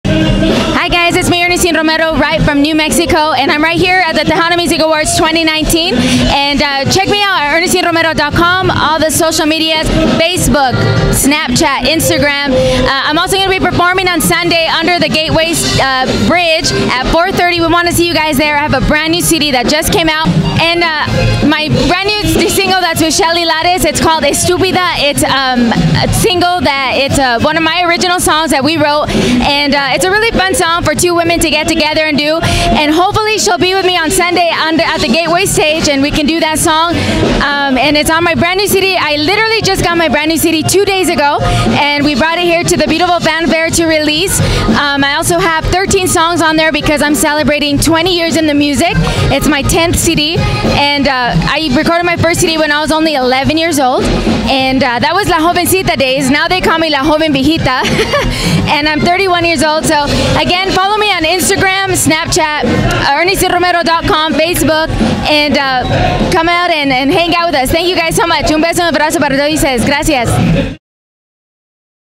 Ernestine Romero speaks on her new album, Shelly Lares duet
Ernestine Romero speaks about her new album, Shelly Lares duet at much more at Tejano Music Awards Fan Fair 2019.